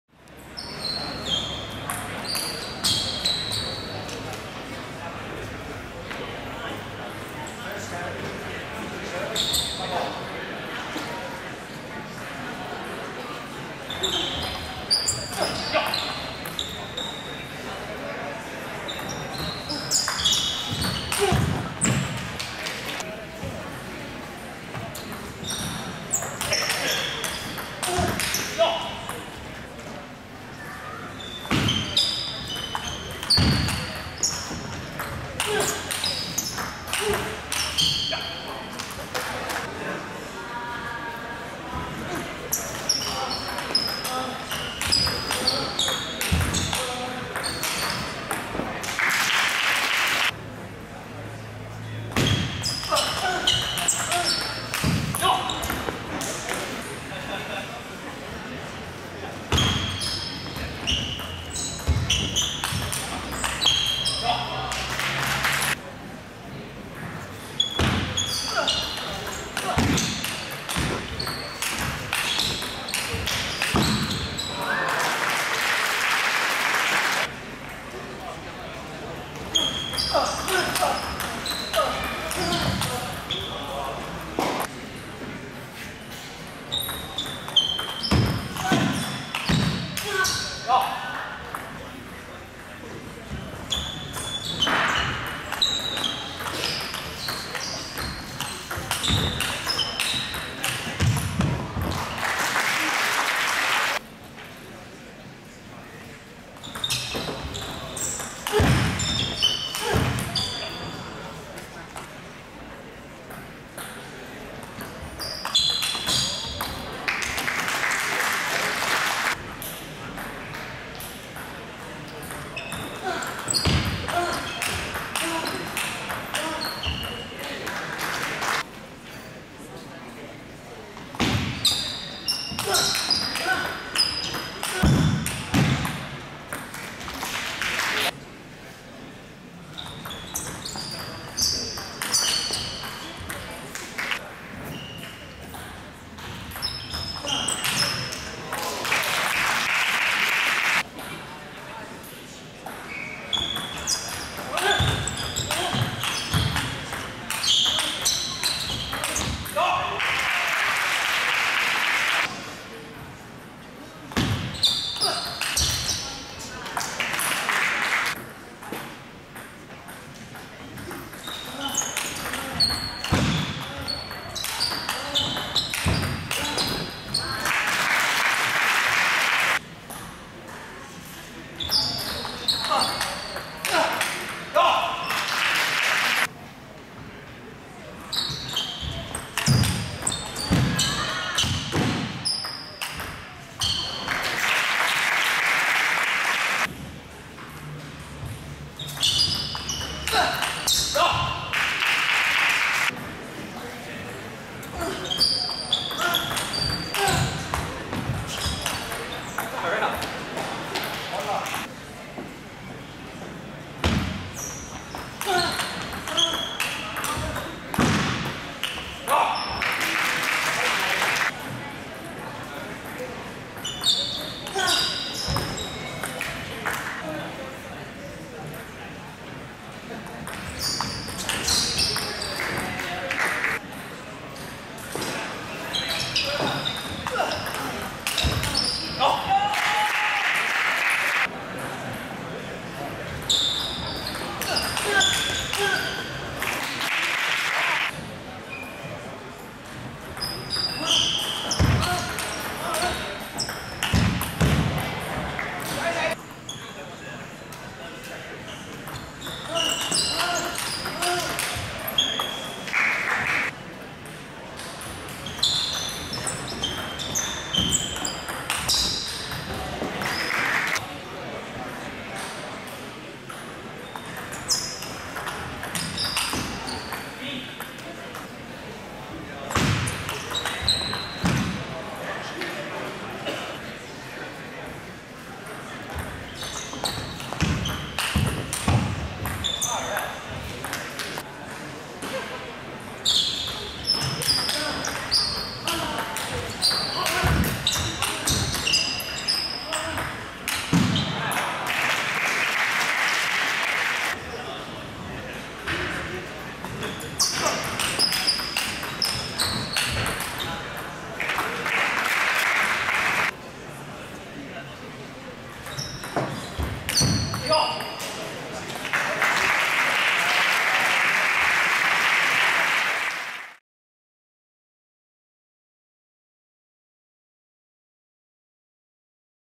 Australian Open 2012 U21 Men's Final - David Powell vs Heming Hu
From the 2012 Australian Open in Brisbane.